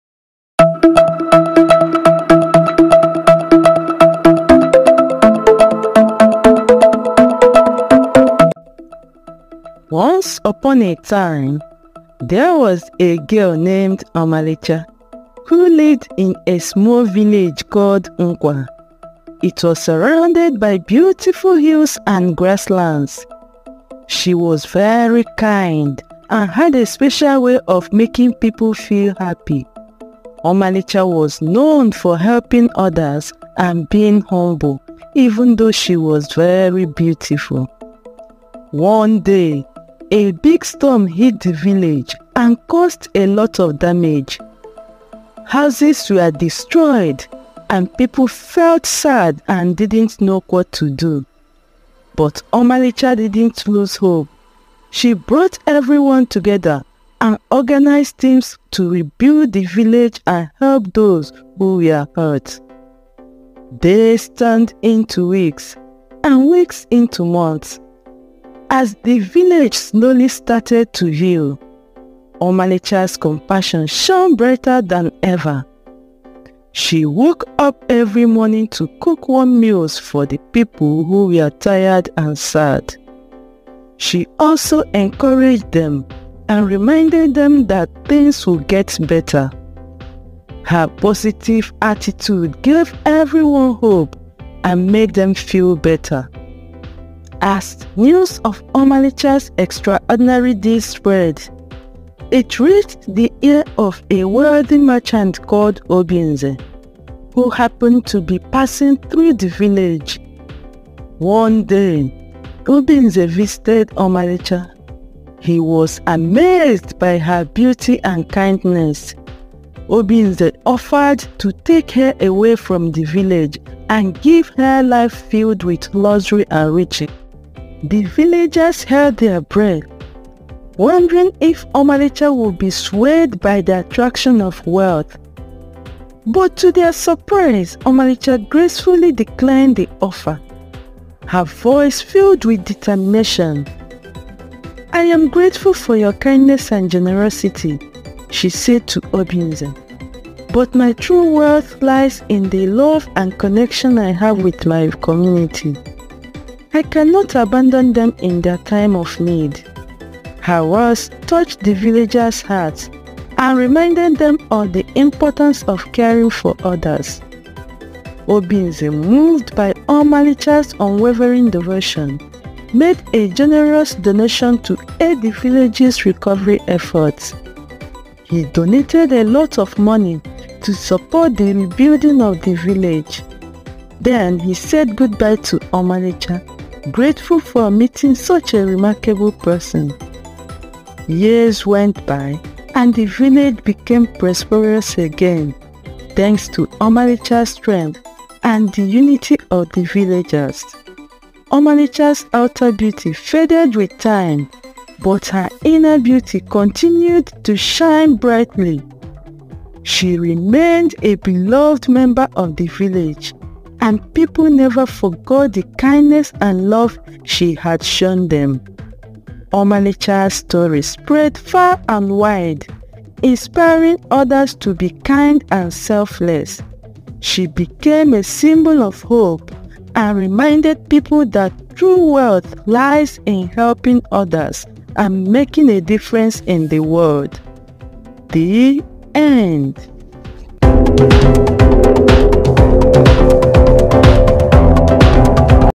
Omalicha the Village Hero
Join the enchanting adventure of "The Village Hero: Omalicha's Tale of Kindness." Follow Omalicha, a selfless girl who brings hope to her village. Experience the transformative power of kindness and genuine connections. This heartwarming story will inspire you to believe in the impact of compassion.